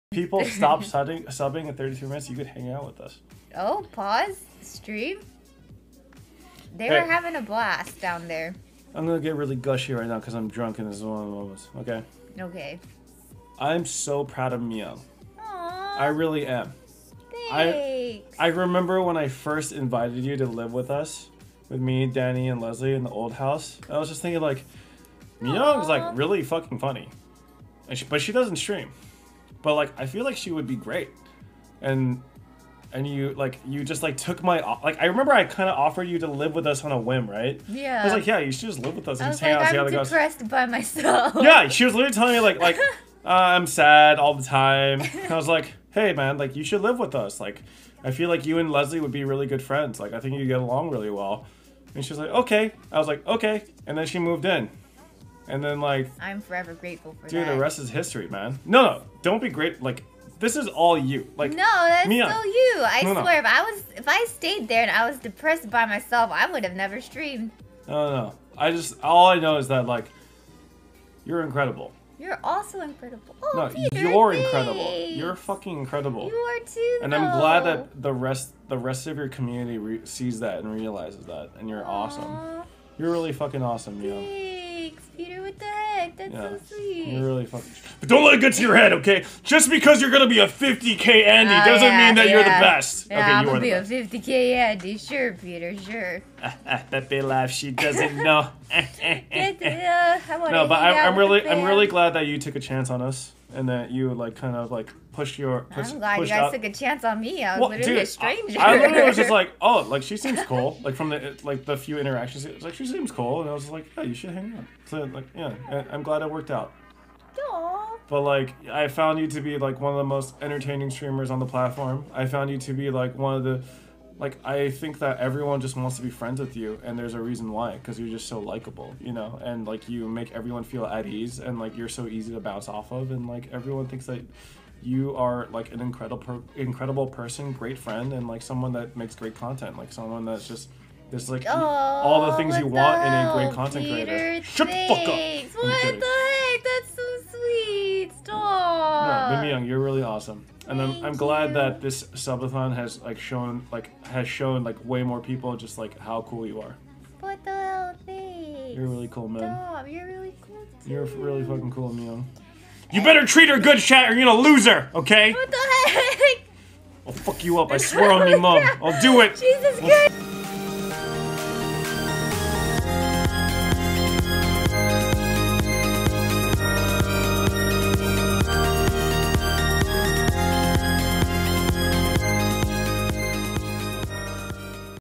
Miyoung trying to hold back tears after Peter's wholesome speech
Aww uncle PP 🥺️